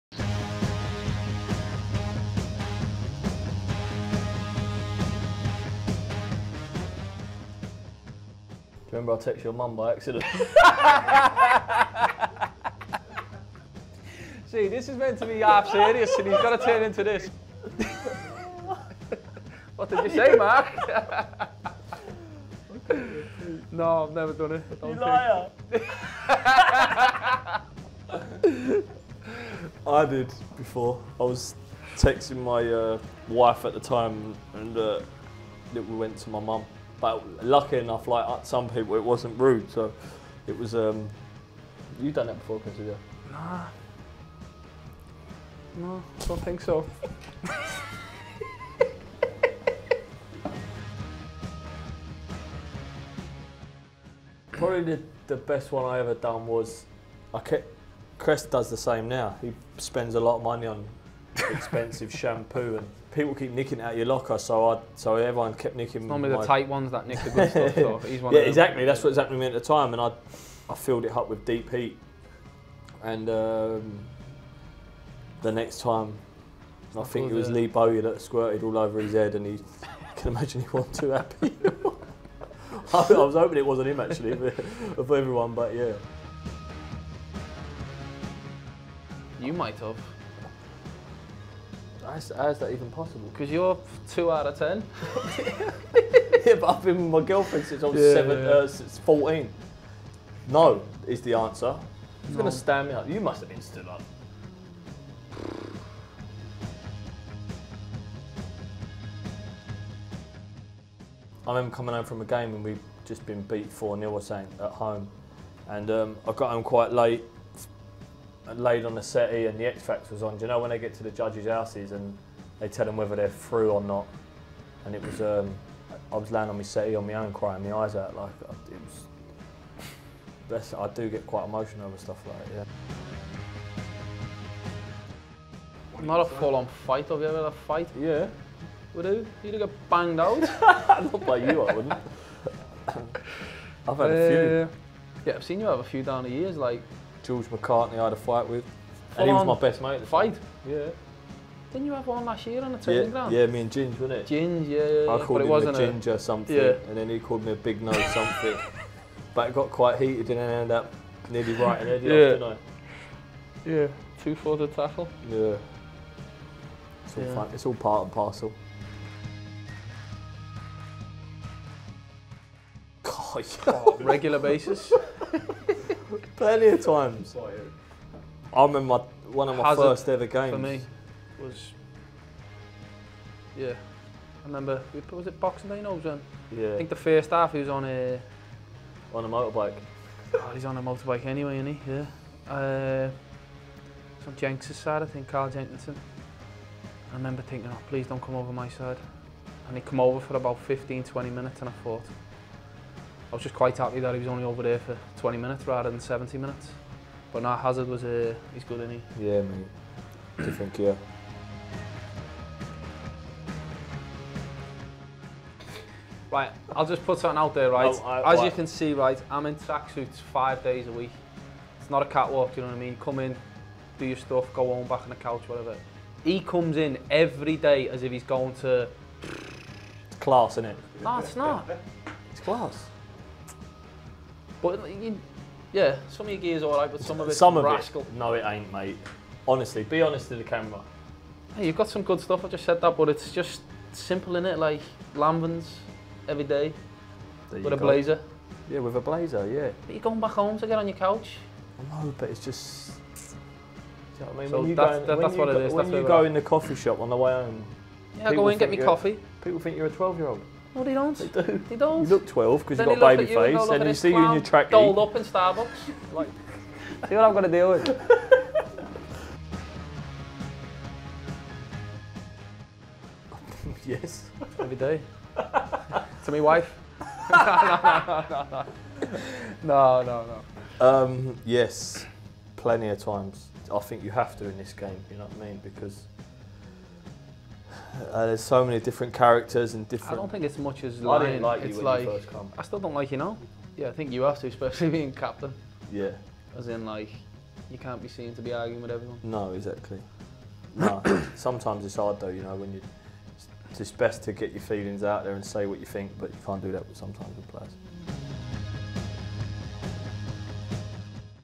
"I texted Cresswell's mum once!" | Mark Noble & Aaron Cresswell | Have You Ever?
We play a game of Have You Ever with West Ham's Mark Noble and Aaron Cresswell! Have they have had a dressing room bust up? Have they ever cried during a film? And have they ever text someone by mistake?